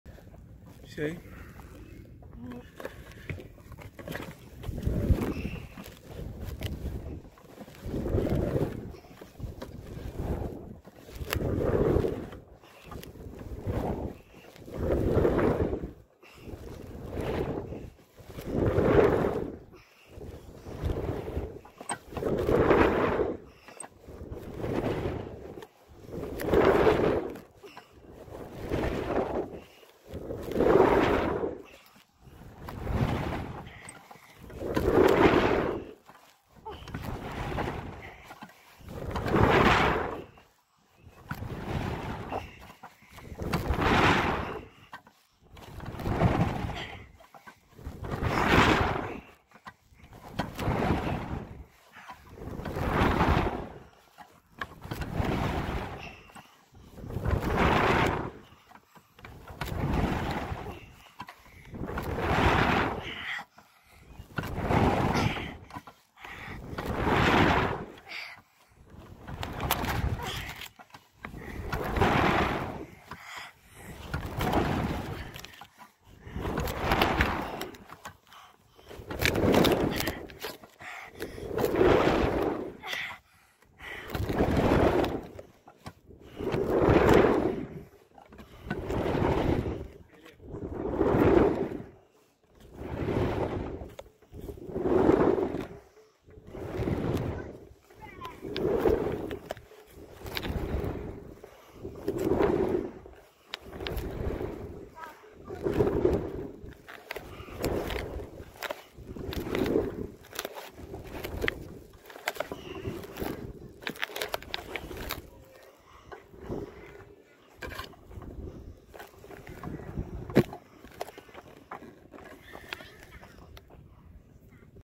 Kiiking - Leo makes a trial run on Kiki - 3.0m
Leonardo tries (in this case, without making a complete turn) the Kiki set at 3.0 meters.